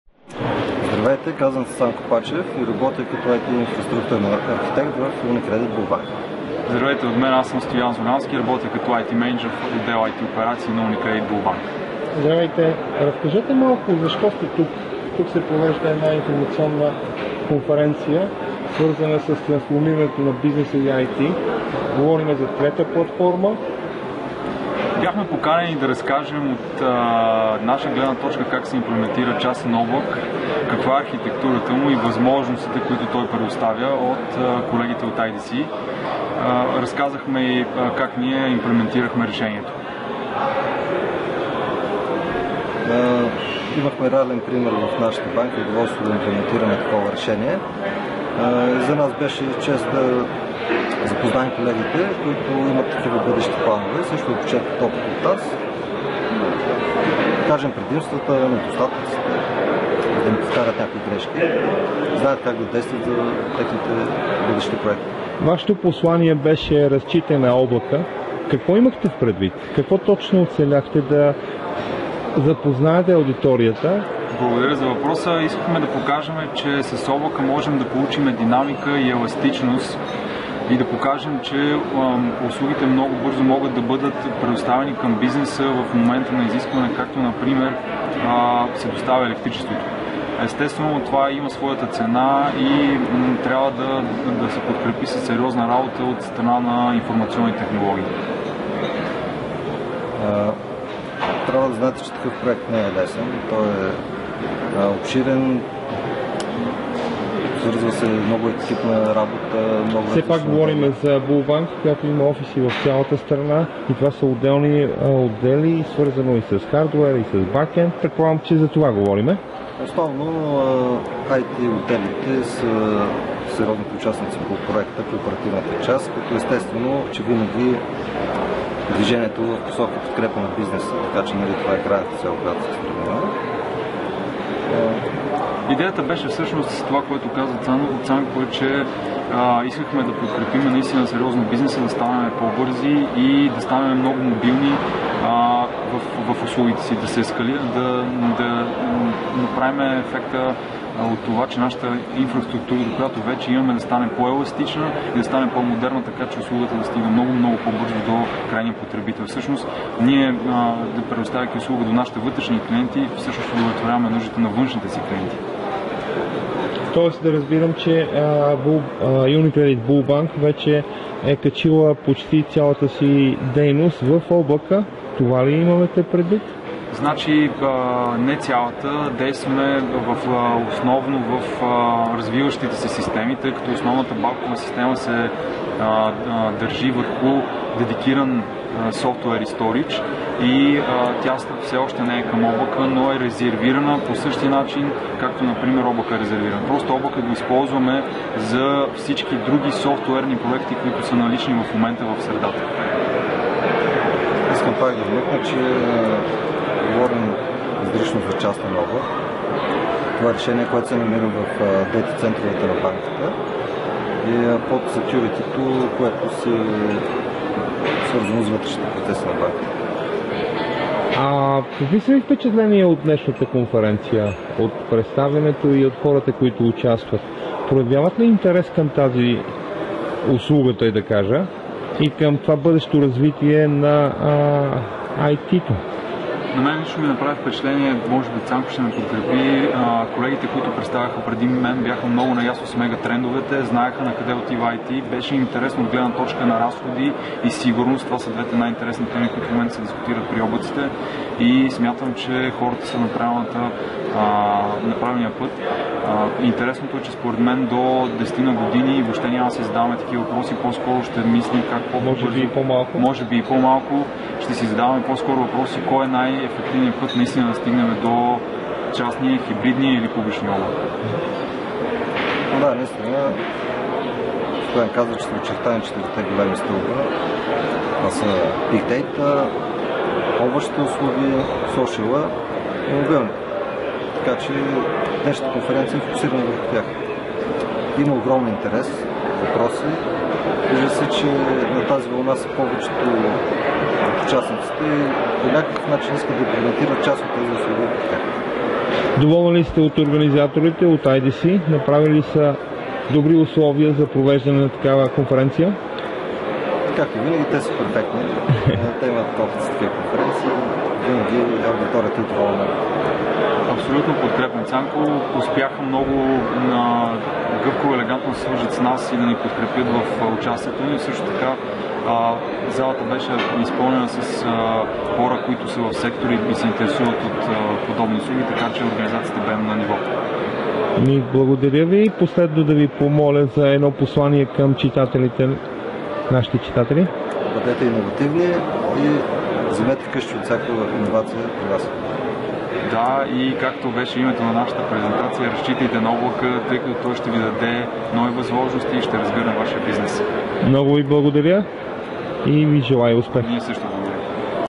Стоян Зулямски и Цанко Пачев от Уникредит Булбанк
Стоян Зулямски и Цанко Пачев от Уникредит Булбанк за ICT: Третата платформа